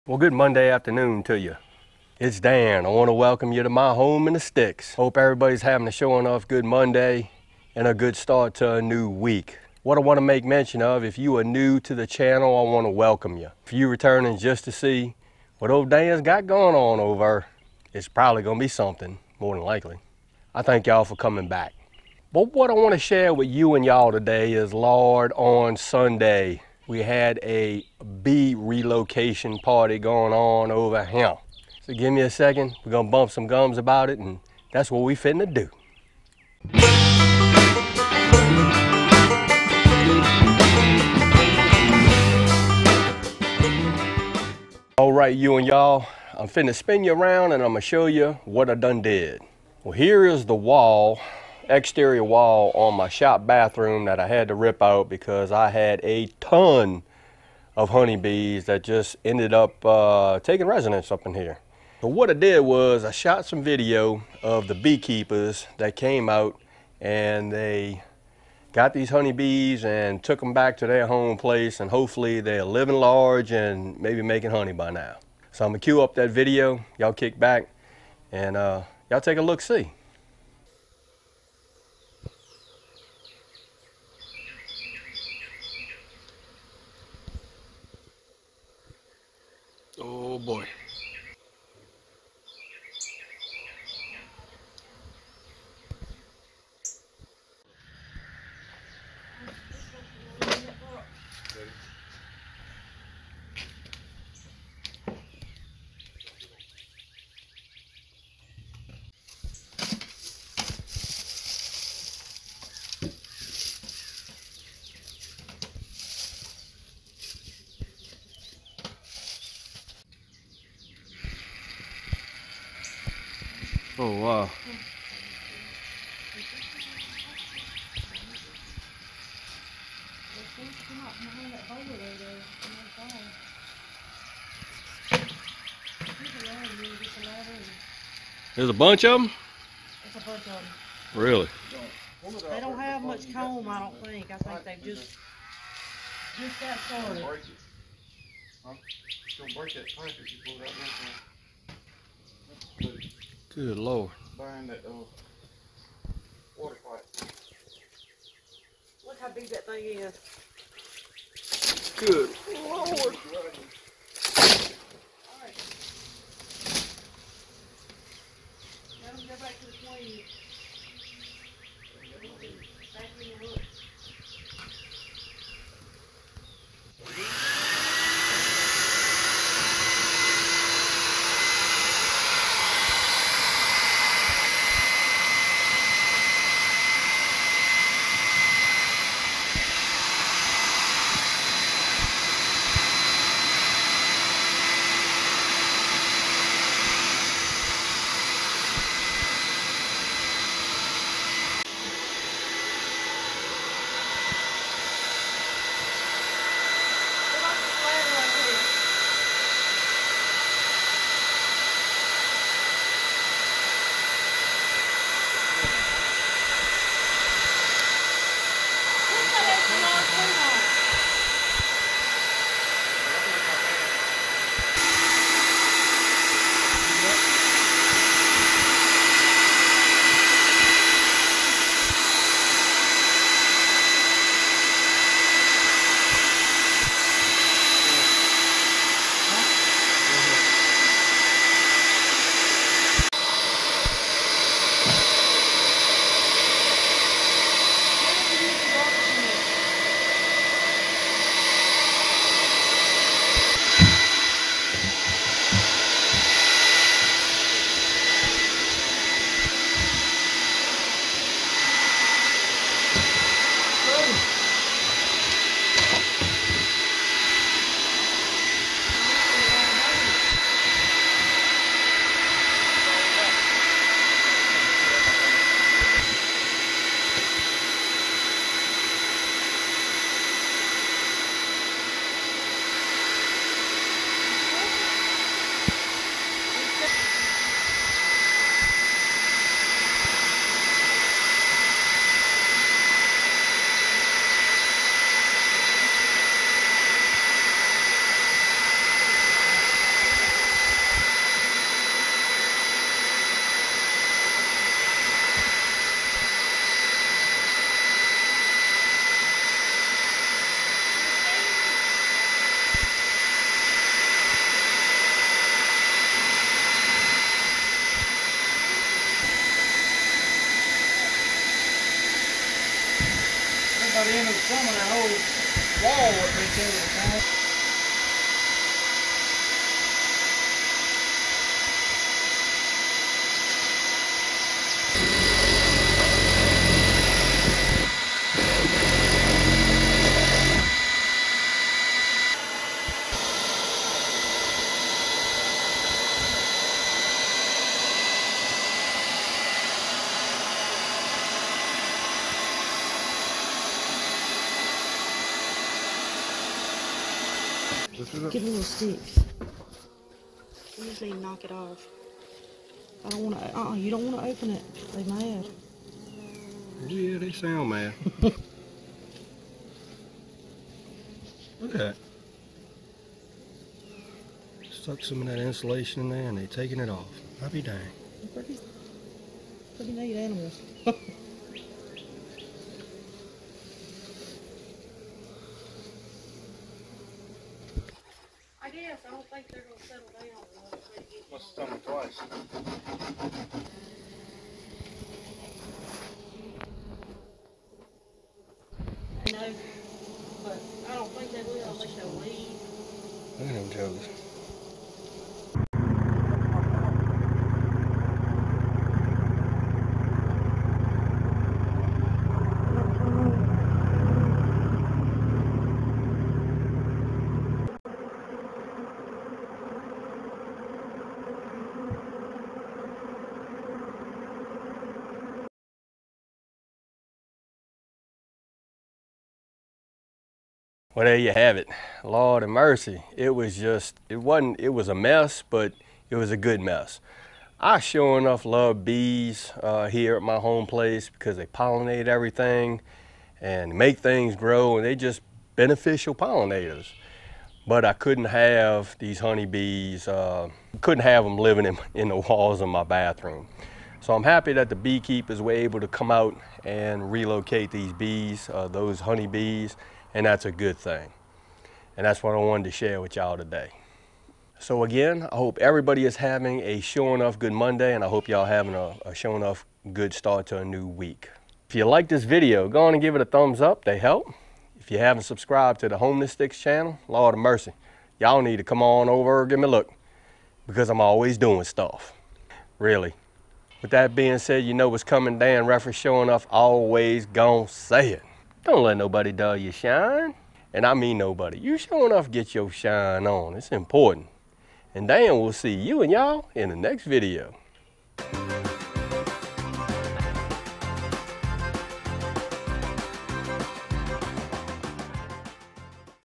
Honey Bee Relocation At The Home In The Sticks
OK y'all, I had some local beekeepers come out to the sticks on Sunday to collect  the honey bees that took up residence in a wall of my shop bathroom!!  Check it out and thank ya for watching!!  Dan